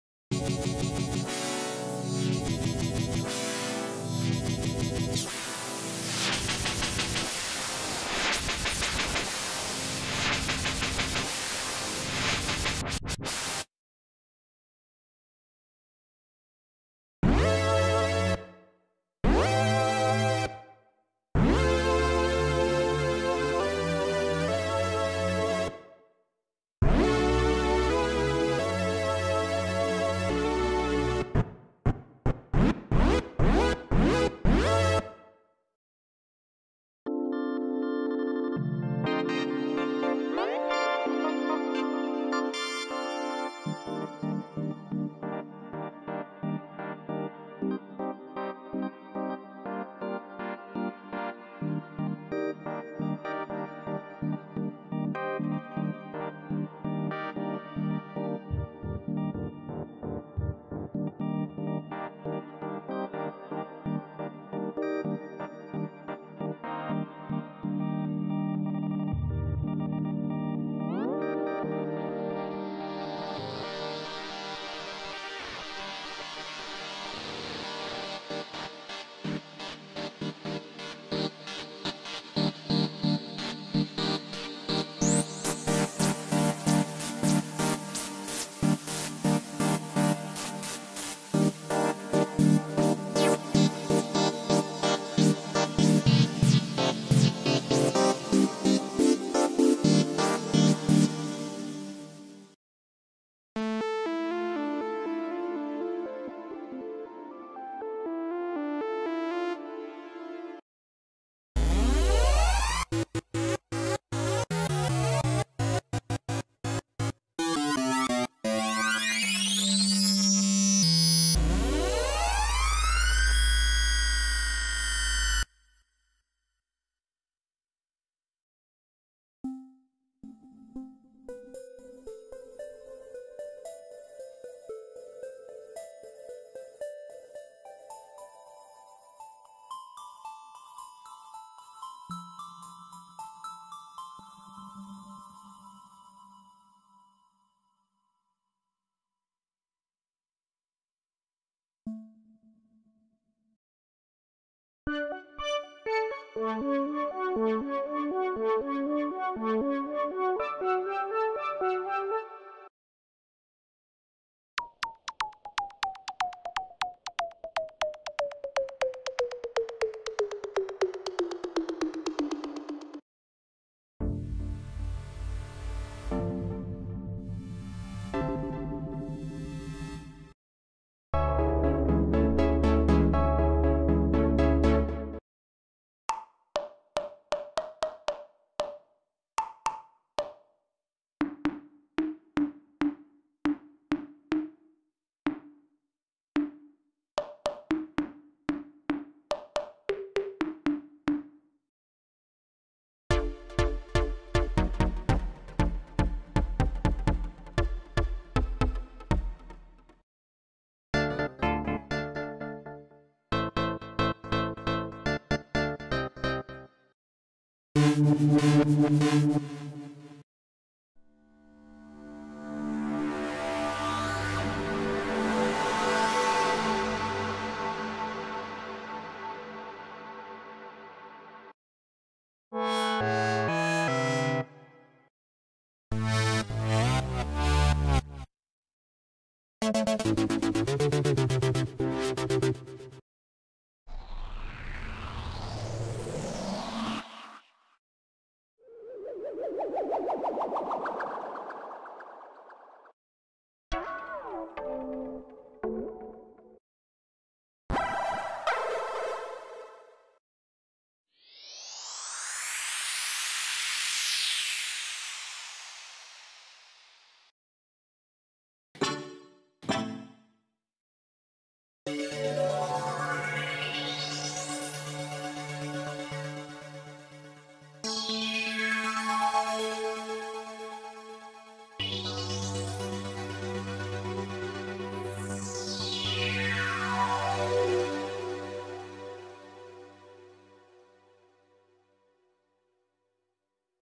Native Instruments | Massive X | Synth Misc Presets
Rethought, rewired, and reincarnated – MASSIVE X is the successor to an iconic synth that helped spawn entire genres. Designed and built from scratch, the new instrument builds on its heritage to rethink and redefine the possibilities of a virtual synth.

Immaculate sound and complete sonic freedom for advanced sound design.

Create with a new level of dynamic, shifting sound, with truly flexible routing and extensive modulation.

Based on the DNA of MASSIVE, built for a new decade and designed to evolve.